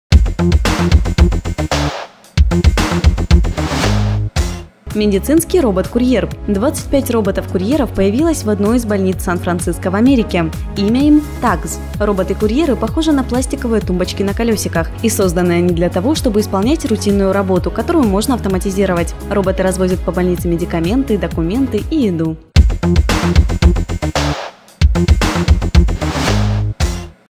Медицинский робот- курьер Новости. Делай Деньги №21
Партнер показа 25 роботов-курьеров появилось в одной из больниц Сан-Фрациско в Америке. Их имя –TUGs. Роботы-курьеры похожи на пластиковые тумбочки на колёсиках. И созданы они для того, чтобы исполнять рутинную роботу, которую можно автоматизировать: роботы развозят по больнице медикаменты, документы,и еду